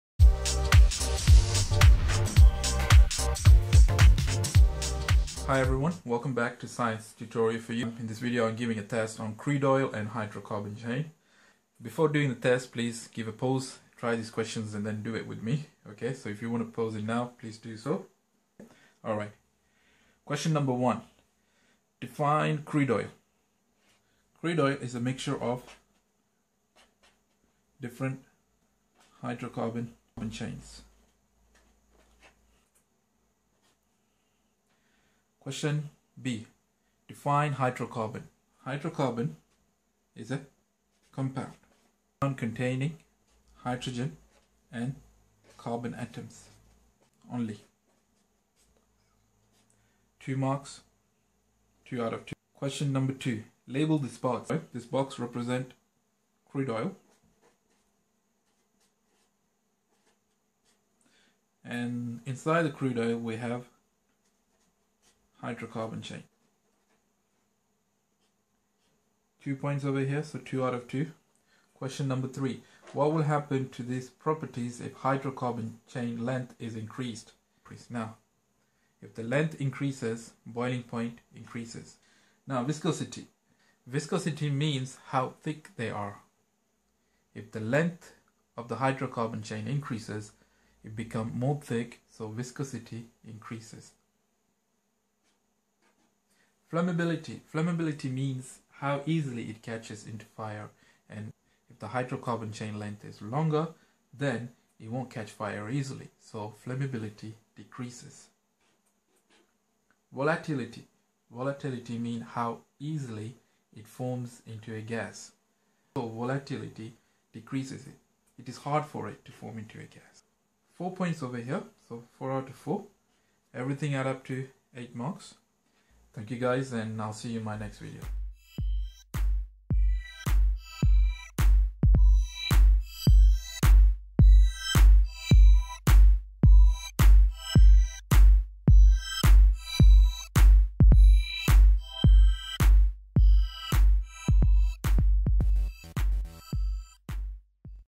Crude Oil and Hydrocarbon Chain. TEST and REVISION
In this video, I am giving a test on crude oil and hydrocarbon chains which acts as good revision for students studying for GCSE chemistry (or O level chemistry). The questions are:

1) What are crude oil and hydrocarbon chain?

2) Label these crude oil and hydrocarbon chain?.

3) What will happen to the properties of hydrocarbon chain when   the length is increased? These properties are boiling point, viscosity or thickness, flammability and volatility.